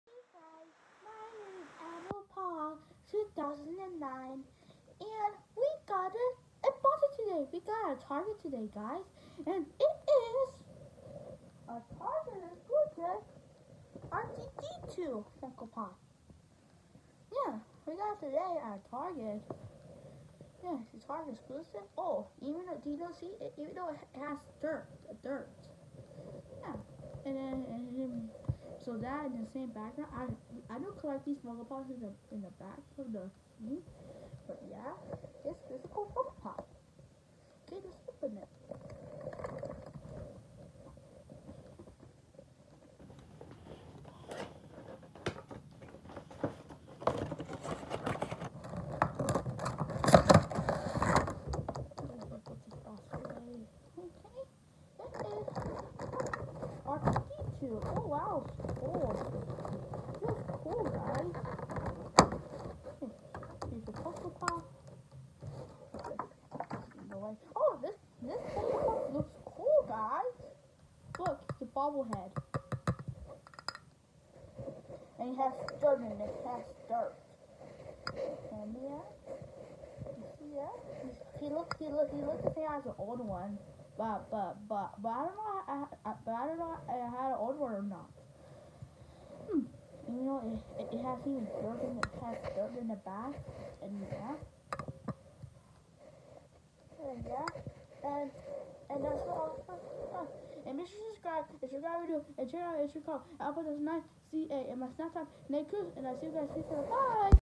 EP2 unboxing: target exclusive R2-D2 funko pop
I got it at target today